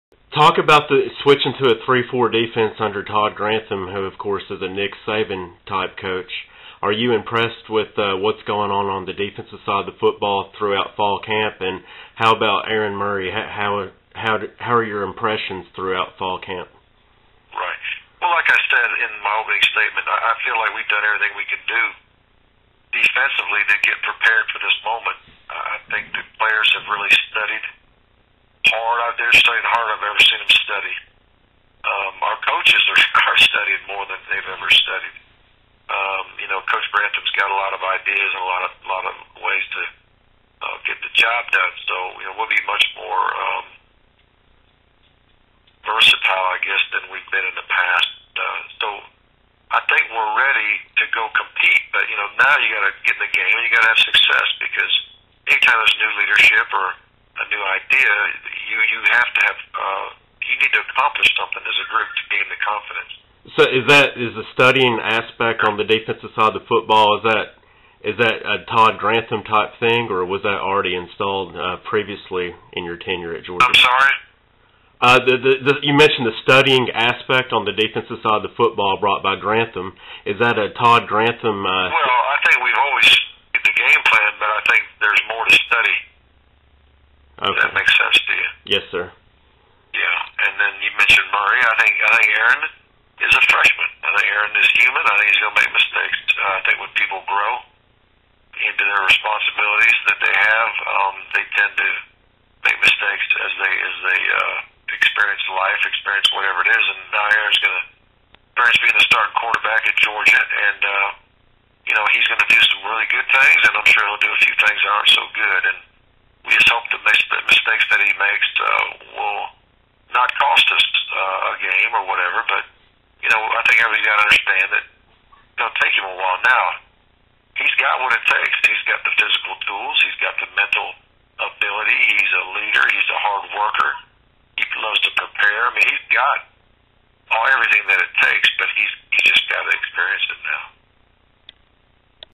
Mark Richt Switching to a 3-4 Defense
Georgia's, Mark Richt, talks about switching to a 3-4 defense for the 2010 season